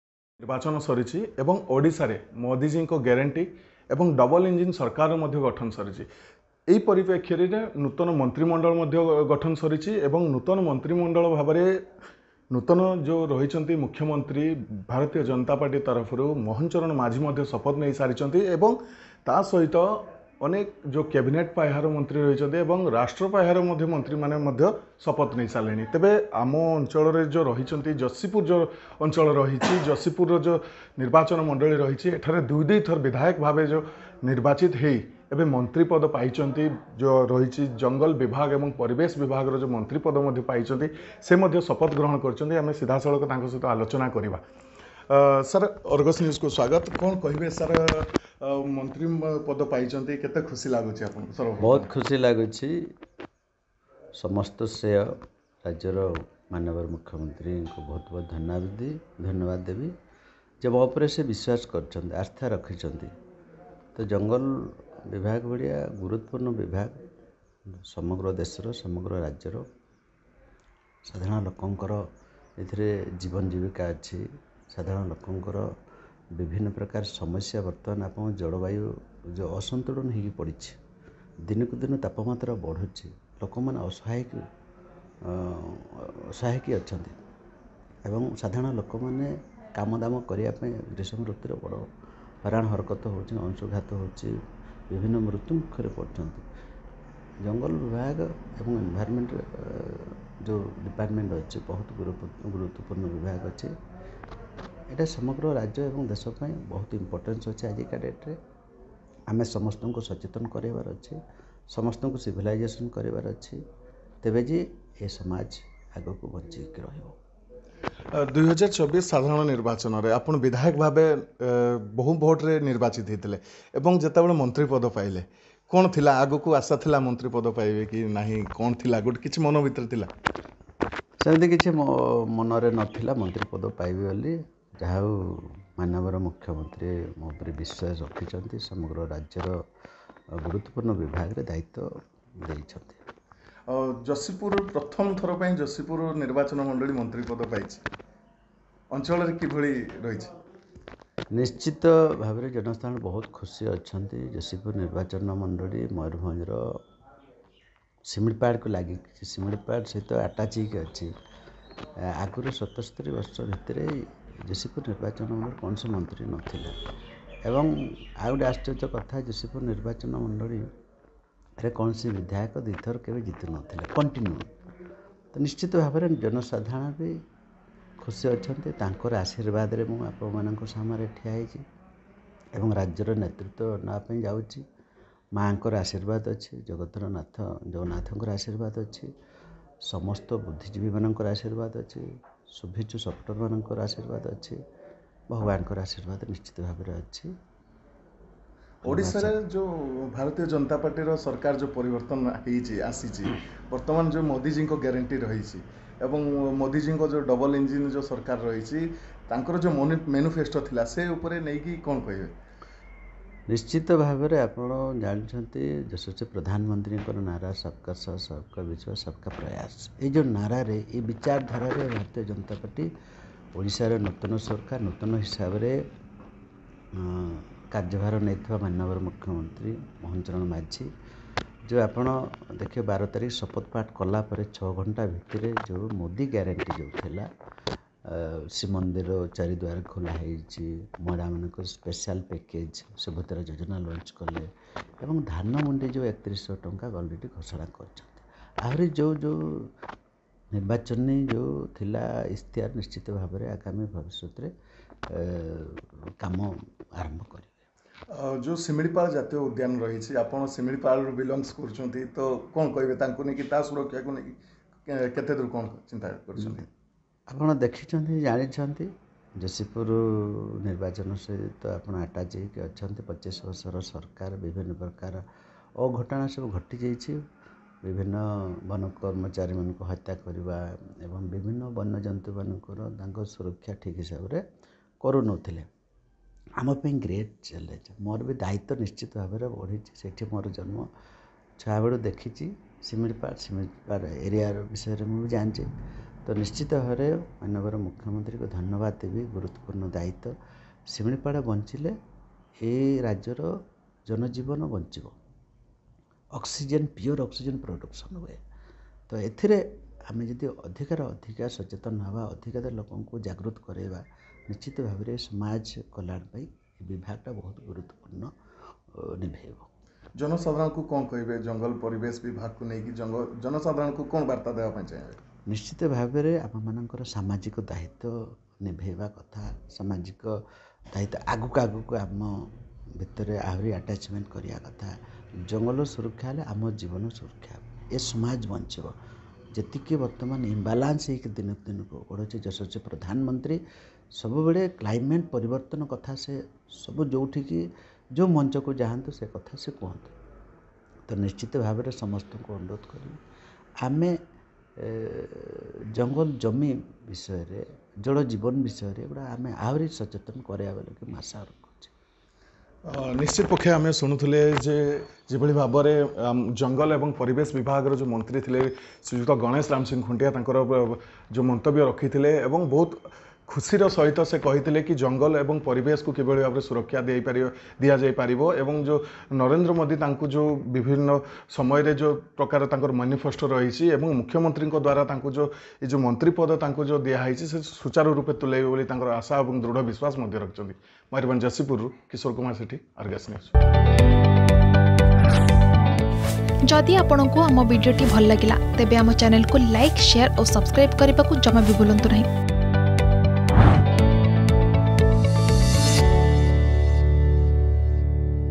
ଜଙ୍ଗଲ,ପରିବେଶ ଏବଂ ଜଳବାୟୁ ପରିବର୍ତ୍ତନ ମନ୍ତ୍ରୀ ହେଲେ ଯଶିପୁର ବିଧାୟକ || Jashipur MLA
"ଜଙ୍ଗଲ, ପରିବେଶ ଏବଂ ଜଳବାୟୁ ପରିବର୍ତ୍ତନ" ଓ "ଶ୍ରମ ଏବଂ କର୍ମଚାରୀ ରାଜ୍ୟ ବୀମା" ମନ୍ତ୍ରୀ ହେଲେ ଯଶିପୁର ବିଧାୟକ ଗଣେଶ ରାମ ସିଂ ଖୁଣ୍ଟିଆ ।